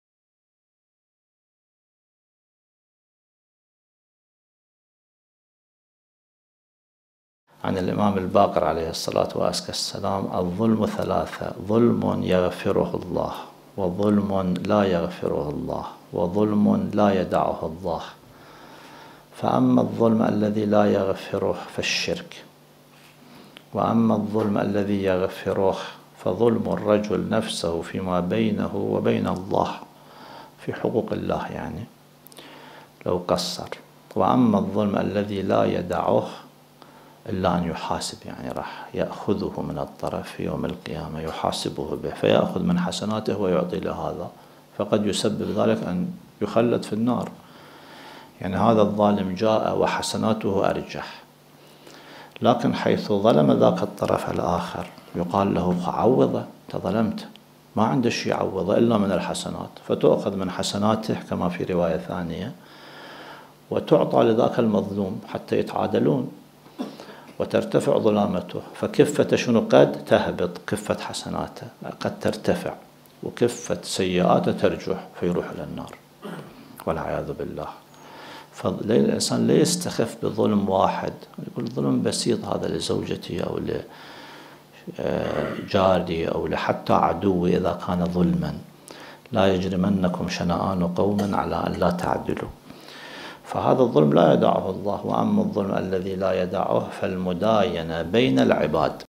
Types of Oppression | Sayed Murtadha Shirazi
In this video, Sayed Murtadha describes the types of oppression using the narration of Imam al-Baqir (as). The oppression is of three types; that which is oppression that is forgiven, that which is not forgiven, and that which is not forgotten.